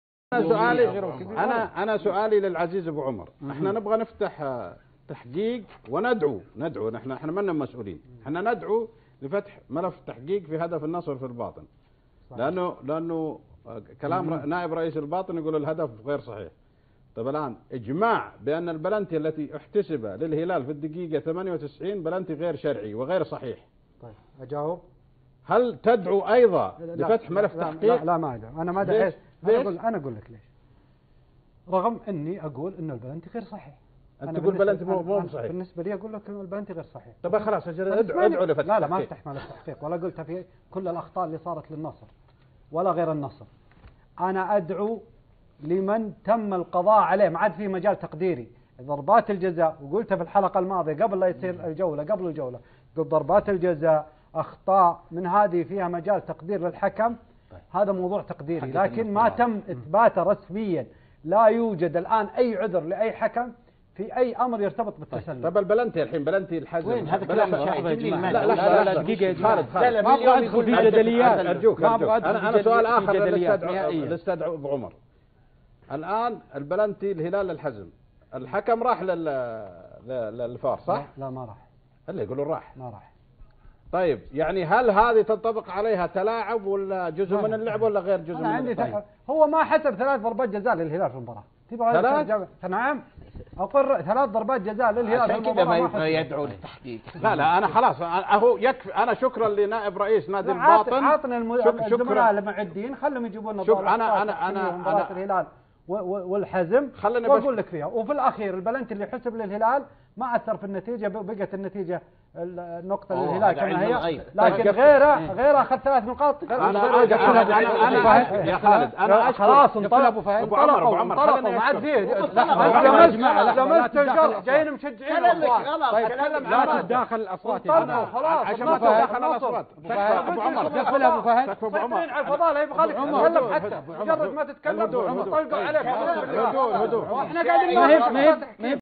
الصرامي للروقي/ نطالب بفتح تحقيق على هدف النصر في الباطن فهل تطالب بذلك في بلنتي الهلال مع الحزم ؟
الصرامي  الروقي  النصر  الهلال  الباطن  الحزم  بلنتي  الفار